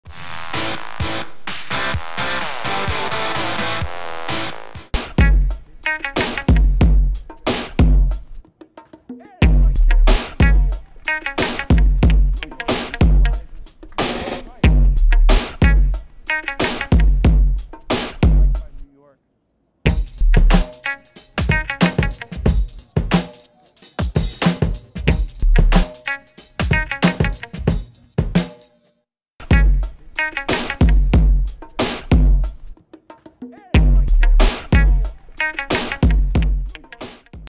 New York Red Bulls: John Rooney free kick goal vs. Chicago
New York Red Bulls midfielder John Rooney scores on a well taken free kick in the club's preseason scrimmage against Chicago Fire at Lockhart Stadium in Fort Lauderdale, FL on February 23, 2011